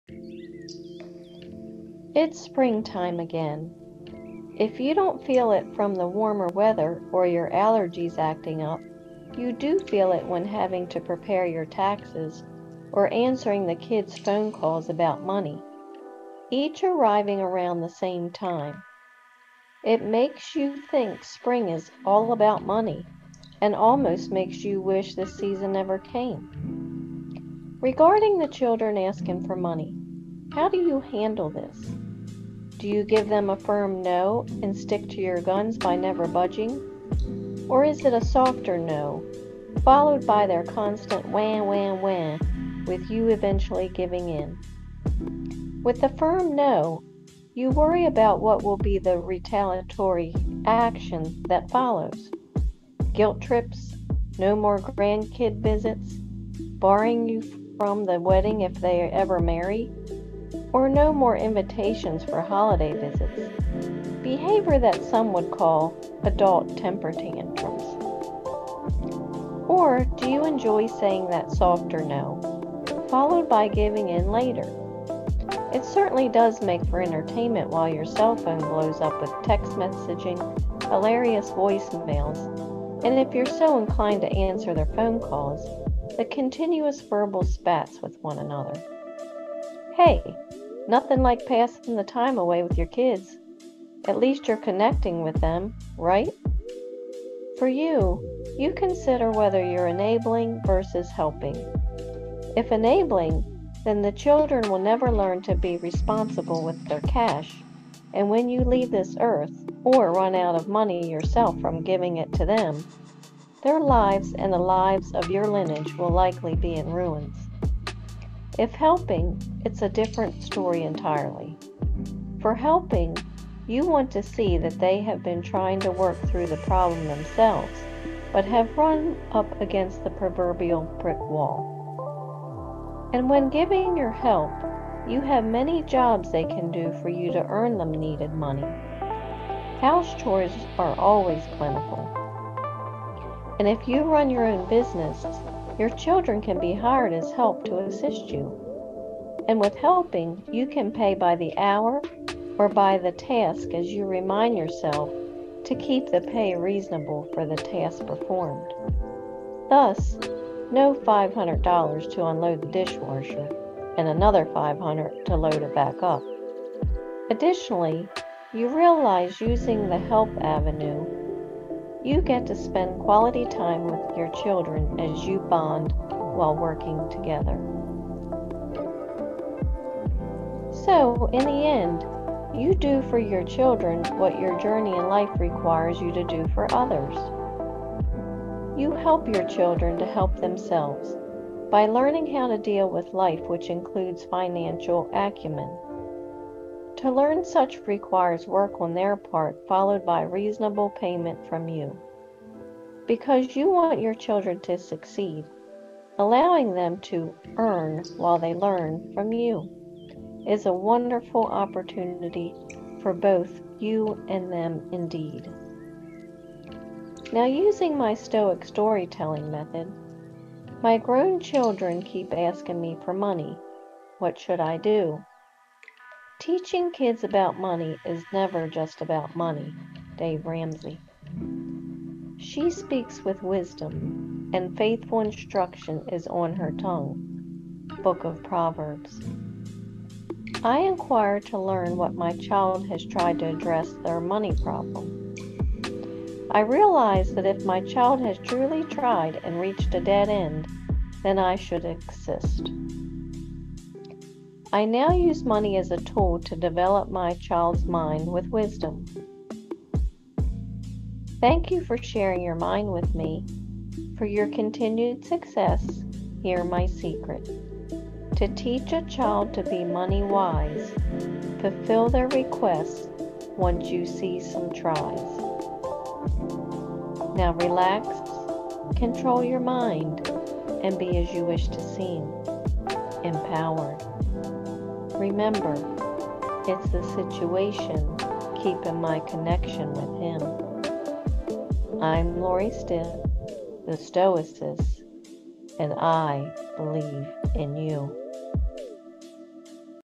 Grown children asking for money
How do you handle your grown children asking you for money? Do you give them a firm “No” and stick to your guns by never budging? Listen to the podcast as you share your wisdom gleaned from training with Stoicess.